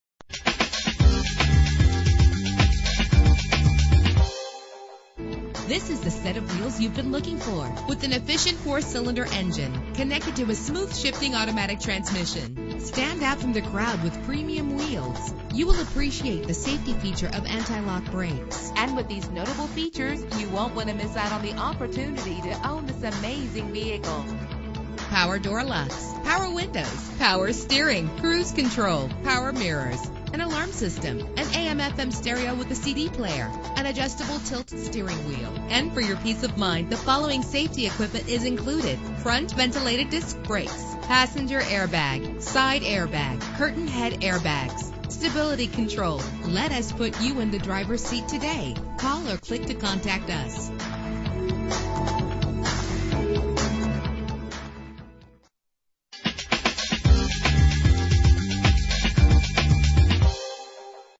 2011 Nissan Juke - Manassas Va
Year: 2011
Make: Nissan
Model: Juke
Trim: S
Engine: 1.6 liter inline 4 cylinder 16 valve
Transmission: Automatic CVT
Color: Sapphire Black
Mileage: 0
Address:
9010 Liberia Avenue